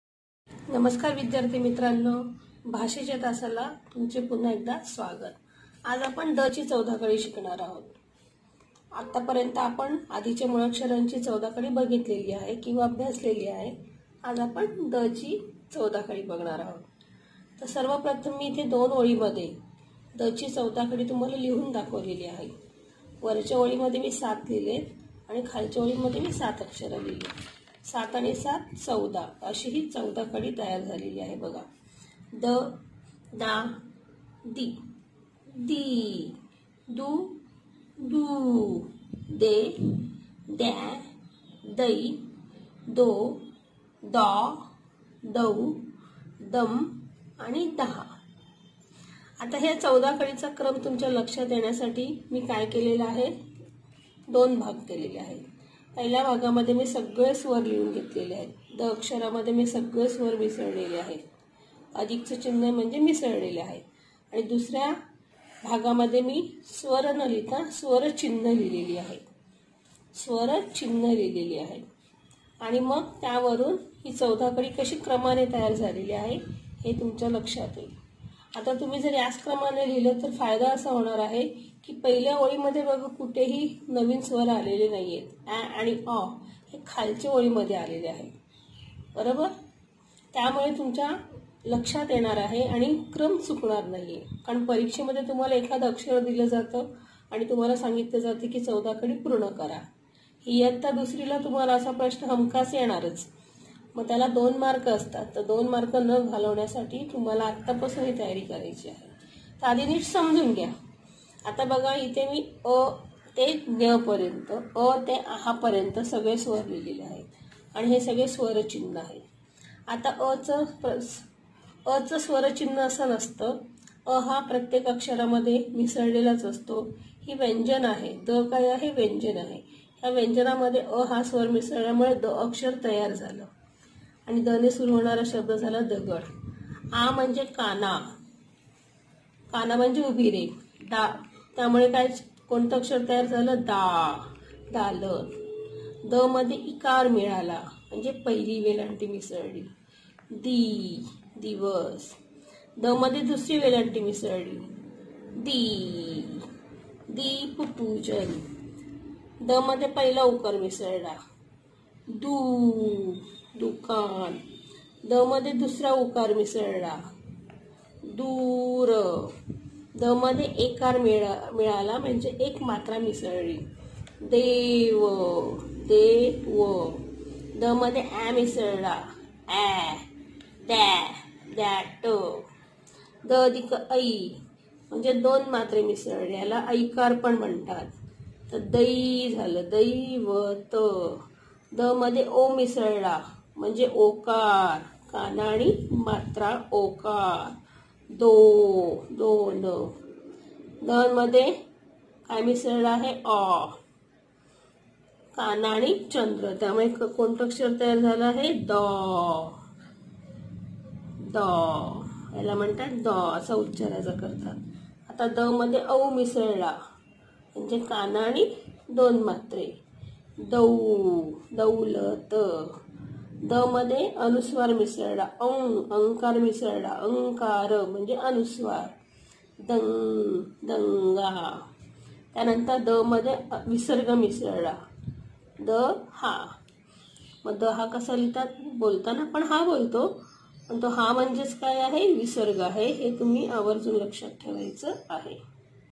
April 22, 2021
द ची चौदा खडी वाचन लेखन सराव करू या.
        इयत्ता - पहिली